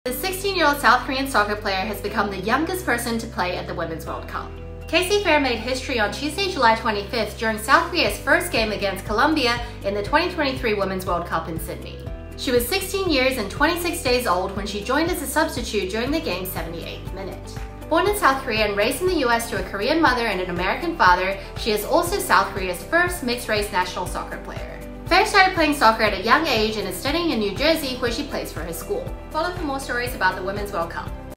This 16-Year-Old South Korean Player Has Become The Youngest Player To Play At The Women's World Cup
Casey Phair, 16, made history on Tuesday, July 25 when she became the youngest player in women’s World Cup history.

As a member of the South Korean national team, Phair entered play during the 78th minute in country’s first game in the World Cup.

This edged out the record was previously held by Nigeria’s Ifeanyi Chiejine record during her debut at 16 years and 34 days old in 1999 by just eight days.

Born in South Korea but raised in the US, Phair is South Korea’s first mixed-heritage player.

Her mother is Korean and her father is American.

Phair started played at a young age and currently plays for the Pingry School while training at the Players Development Academy (PDA) in New Jersey.

The young soccer star recently played for the South Korean Under-17 Team back in April, when she helped the team qualify for the 2024 AFC Under-17 Women’s Asian Cup by scoring two goals against Tajikistan and three goals against Hong Kong.

Shortly afterward, Phair was drafted to the senior team for this year’s World Cup.

“[Phair] is the future of this team. We need strong, fast players with physicality,” South Korea’s team manager Colin Bell said.

Despite the team’s 2-0 loss against Colombia, Bell remains optimistic.

“We’re still in the competition. We still have chances to progress but we need more intensity. We need that at club level in South Korea, need that type of player,” he said.